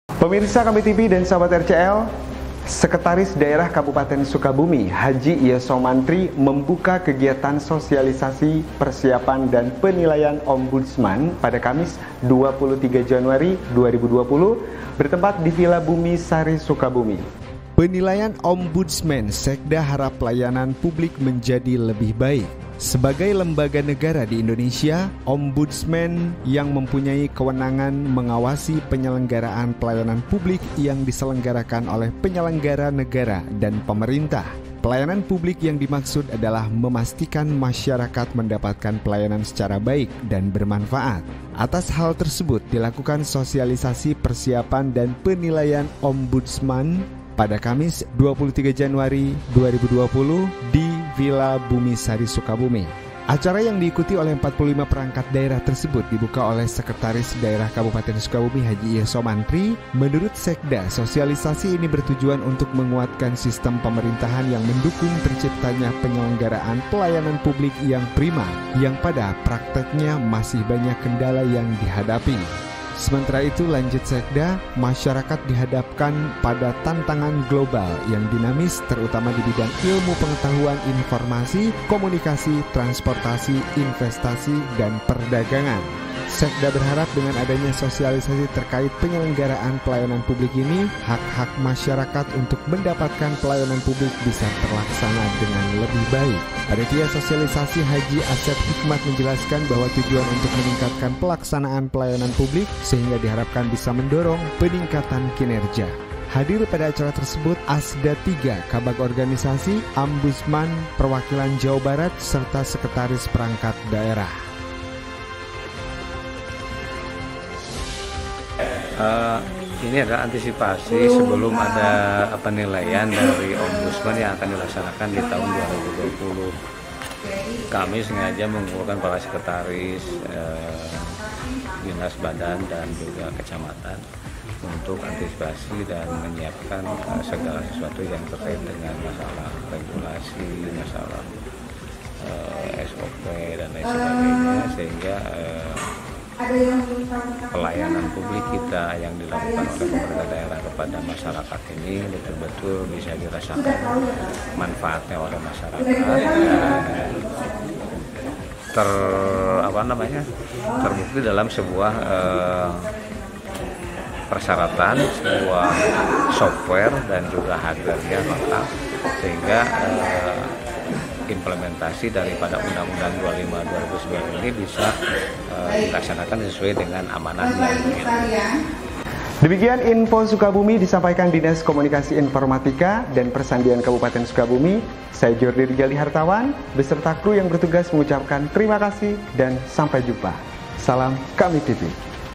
PENILAIAN OMBUDSMAN, SEKDA HARAP LAYANAN PUBLIK MENJADI LEBIH BAIK
Sebagai lembaga negara di Indonesia, Ombudsman yang mempunyai kewenangan mengawasi penyelenggaraan pelayanan publik yang diselenggarakan oleh penyelenggara negara dan pemerintahan.

Pelayanan publik yang dimaksud adalah memastikan masyarakat mendapatkan pelayanan secara baik dan bermanfaat.

Atas hal tersebut dilakukan sosialisasi Persiapan dan Penilaian Ombusdman pada Kamis, (23/1)di Villa Bumi Sari Sukabumi.

Acara yang diikuti oleh 45 Perangkat Daerah tersebut dibuka oleh Sekretaris Daerah Kabupaten Sukabumi, H. Iyos Somantri. Menurut Sekda Sosialisasi ini bertujuan untuk menguatkan sistem pemerintahan yang mendukung terciptanya penyelenggaraan pelayanan publik yang prima yang pada prakteknya masih banyak kendala yamg dihadapi

"penyelenggaraan publik masih dihadapkan pada masalah yang belum sesuai dengan kebutuhan dan perubahan. Hal tersebut bisa disebabkan oleh belum siapnya menanggapi dampak berbagai masalah pembangunan yang komplek" ungkapnya

Sementara itu, lanjut Sekda,  masyarakat dihadapkan pada tantangan global yang dinamis terutama dibidang ilmu pengetahuan informasi, komunikasi,  Trasportasi, Investasi dan perdagangan

Sekda berharap dengan adanya sosialisasi terkait penyelenggaraan pelayanan publik ini, hak hak masyarakat untuk mendapatkan pelayanan publik bisa terlaksana dengan lebih baik.

Panitia Sosialisasi H. Asep Hikmat menjelaskan bahwa tujuan untuk meningkatkan pelaksanaan pelayanan publik sehingga diharapkan bisa mendorong pemingkatan kinerja

Hadir pada acara tersebut
Assda III, Kabag Organisasi, Ombusdman Perwakilan Jawa Barat serta Sekertaris Perangkat Daerah